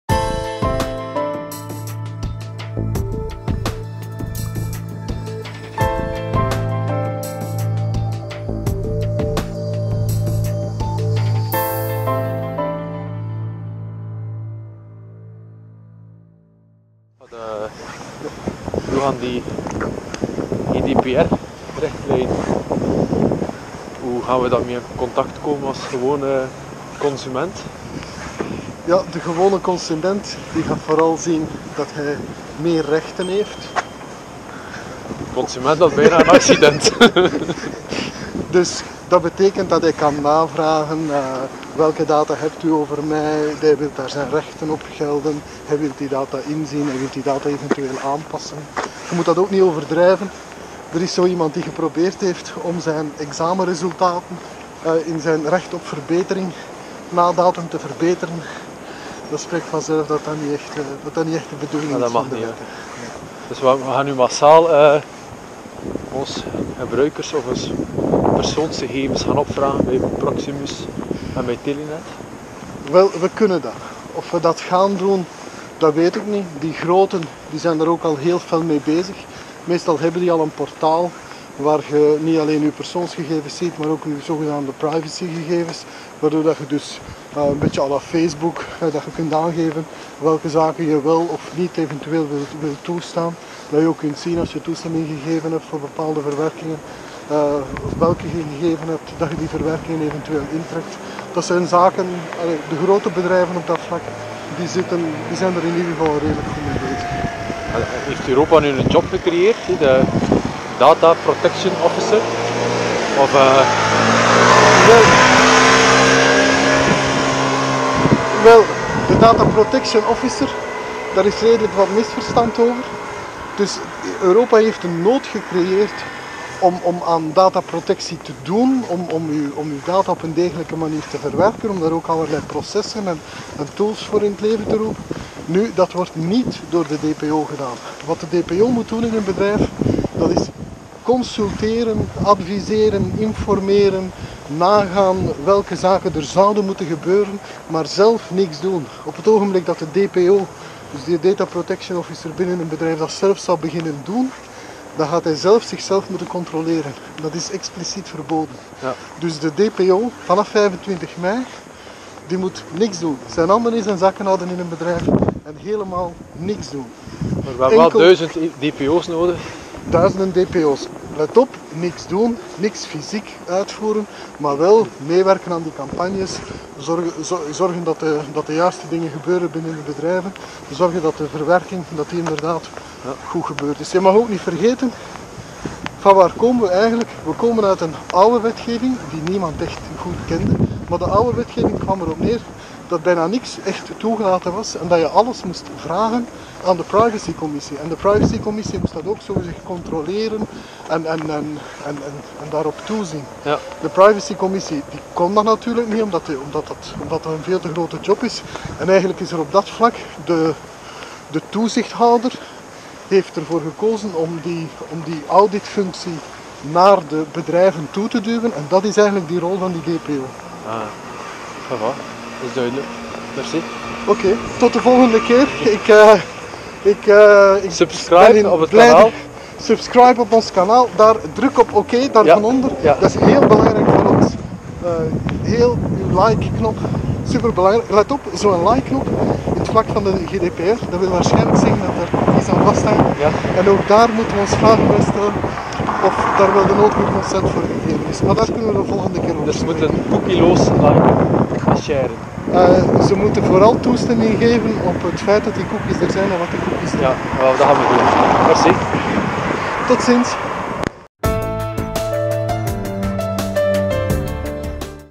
Gene wandelt door de week met Johan Galle #1
Deze week sprak ik met Johan Galle over de GDPR.